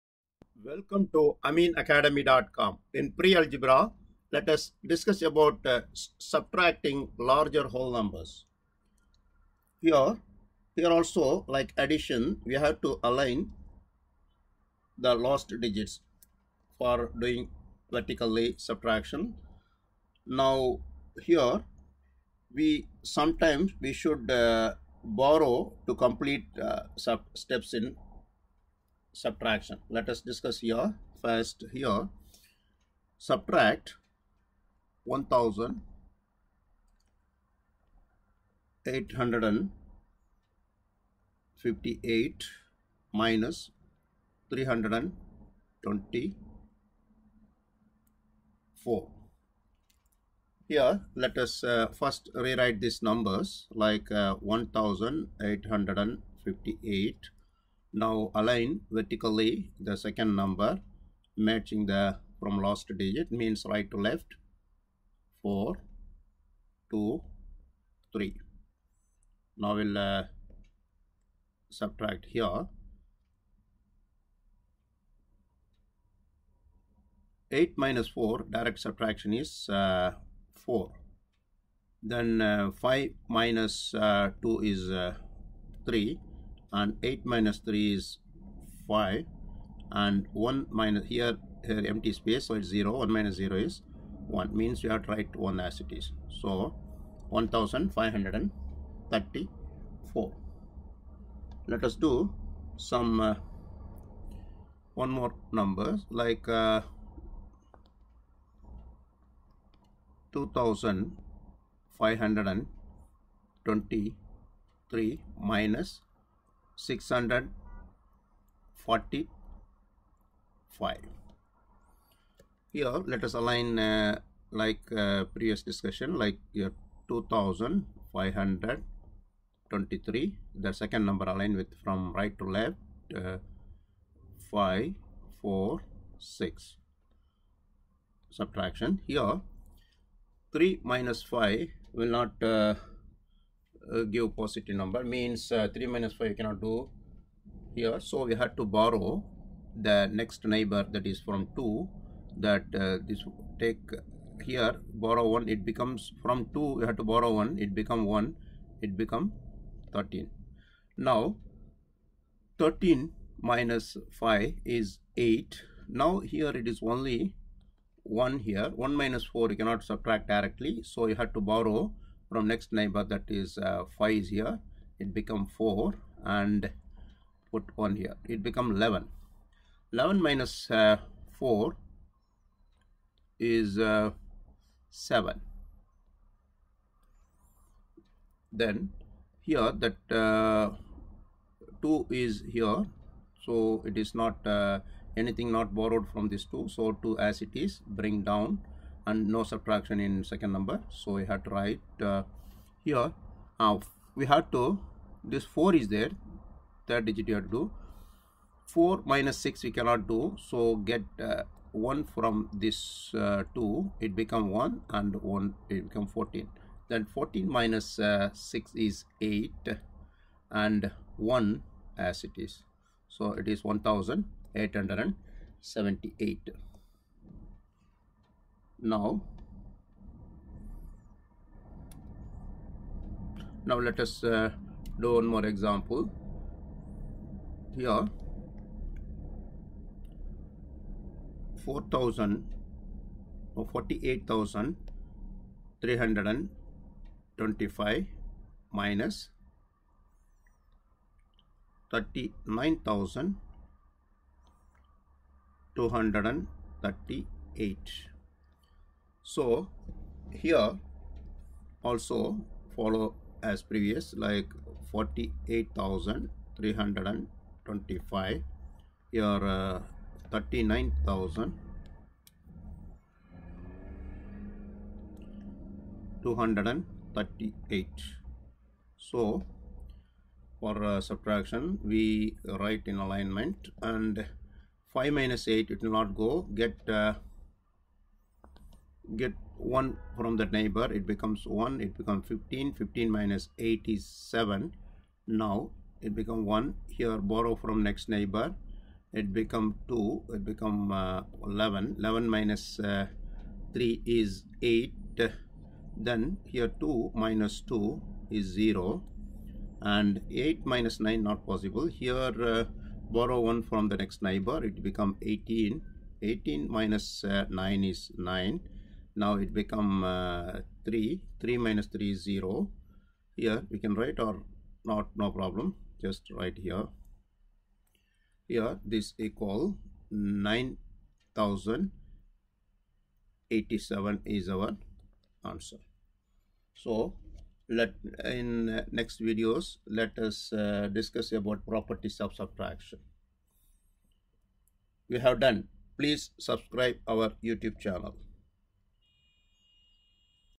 Pre algebra : - ( Subtracting larger whole numbers ; Solving problems ) - 10.
Pre- algebra is a common name for a course in middle school level math. The objective of it is to prepare students and readers for the study of algebra.
The whole numbers are positive numbers, including zero, without any decimal or fractional parts. Subtracting larger whole numbers ; Solving problems .